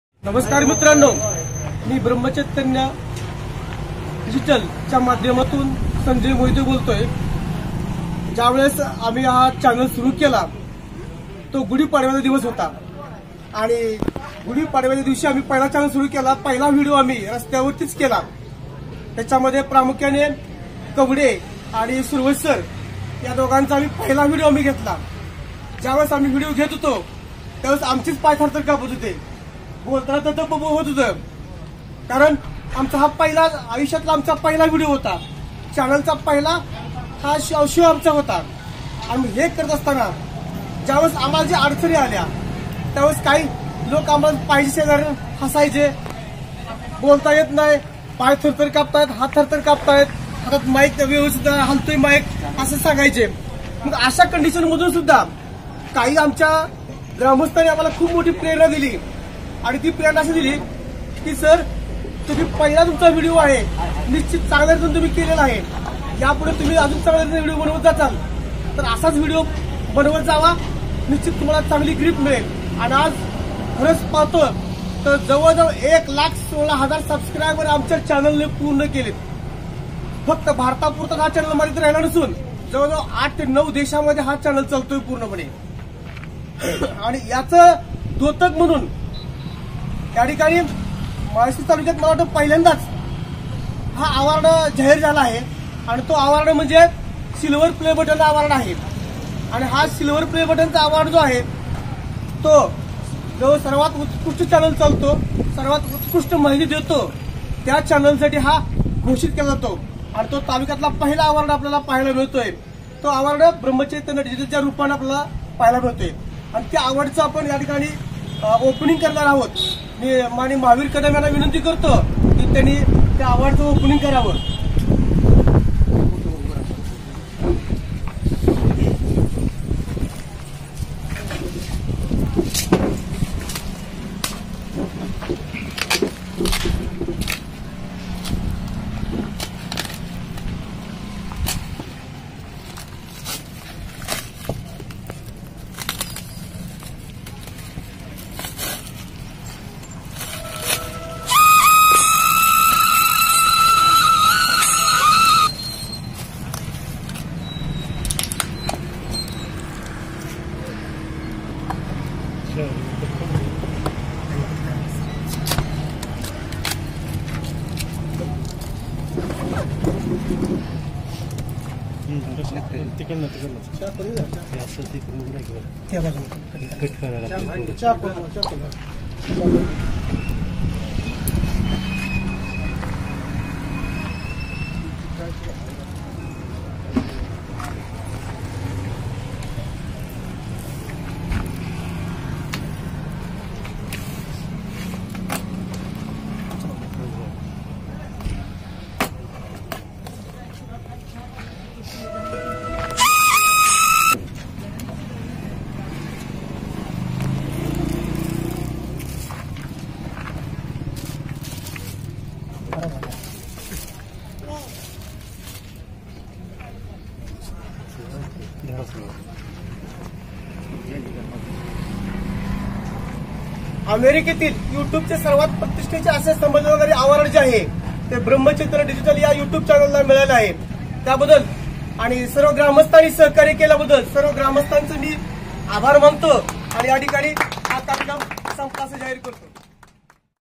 ब्रह्मचैतन्य डिजिटल ला मिळाले अमेरिकेतून सिल्वर प्ले बटन
ब्रह्मचैतन्य डिजिटल या चॅनल ला 100000 सबस्क्राईब टप्पा पूर्ण केल्याबद्दल युट्युब चॅनेल सिल्वर प्ले अवॉर्ड दिला असून ग्रामस्थांच्या सहकार्याने या आवाजाचे अन बॉक्सिंग करण्यात आले त्यावेळी सम्राट शिक्षण संस्थेचे अध्यक्ष विष्णू काकडे महावीर कदम पारसे सर तसेच किरण सोलंकर व ग्रामस्थ उपस्थित होते.